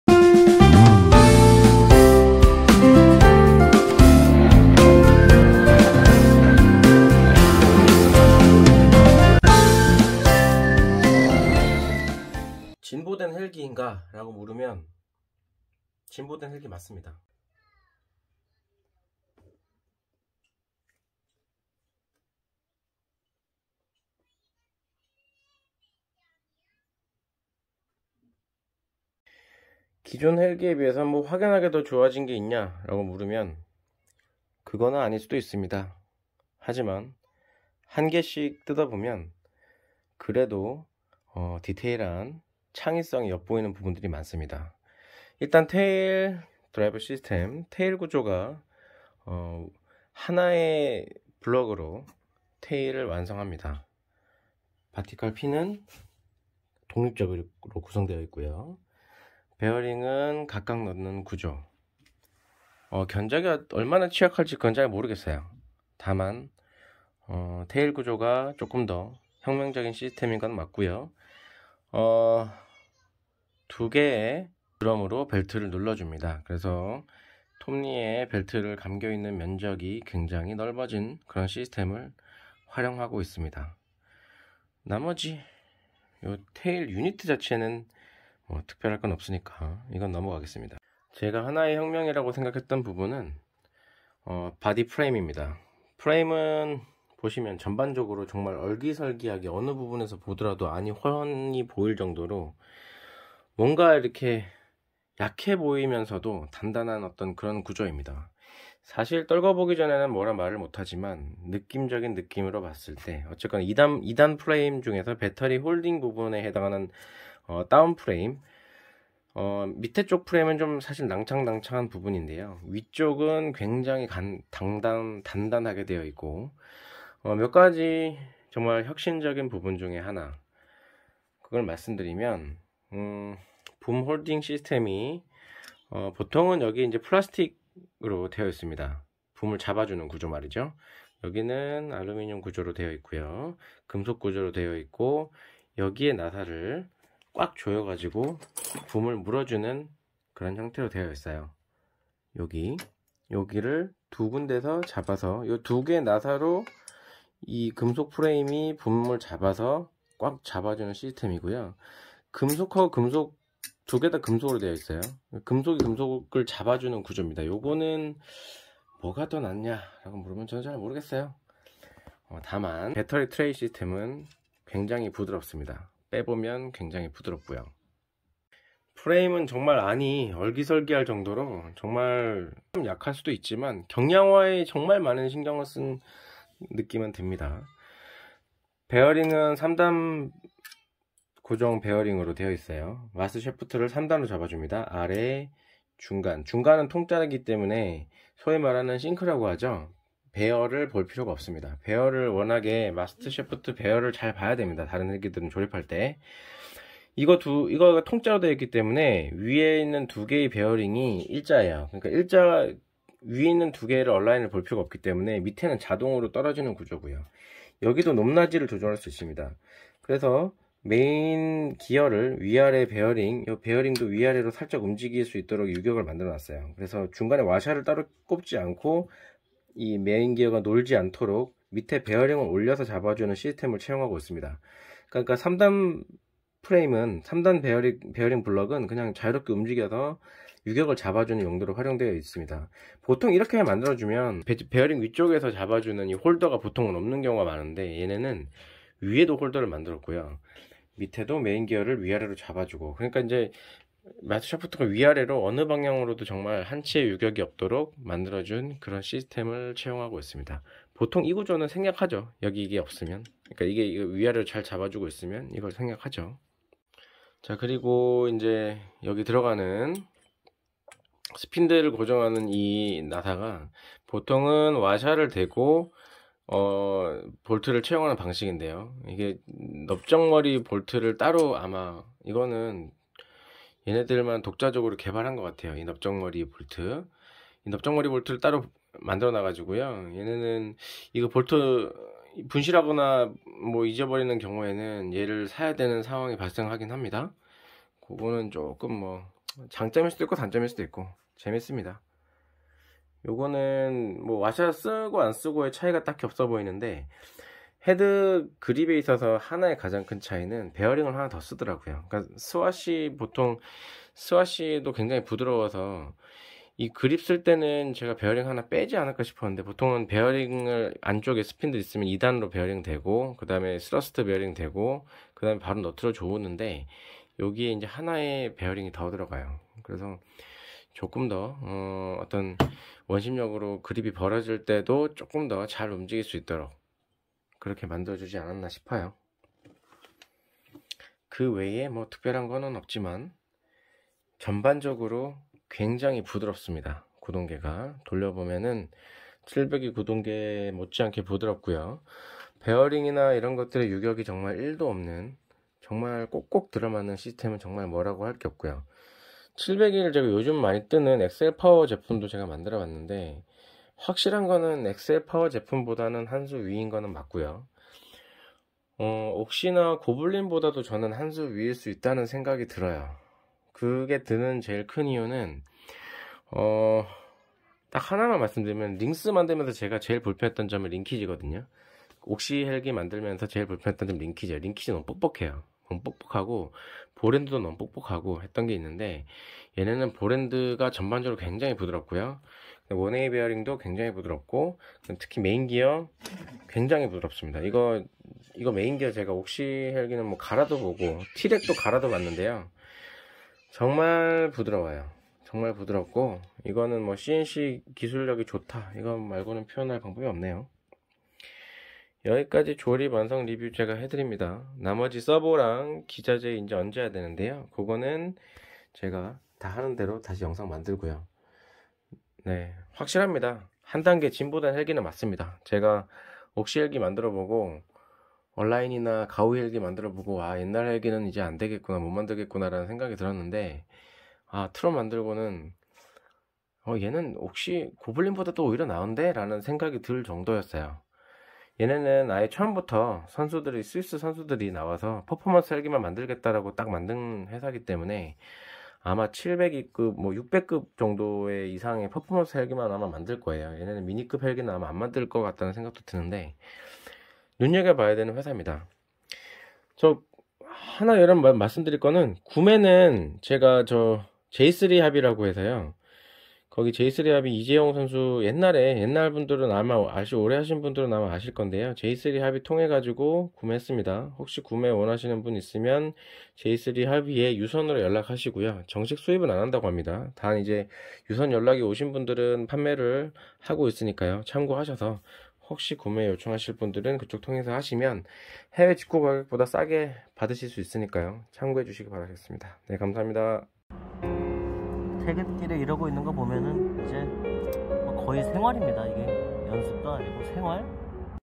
Tron 7.0 조립후기!
조립이 너무나 쉬운 트론헬기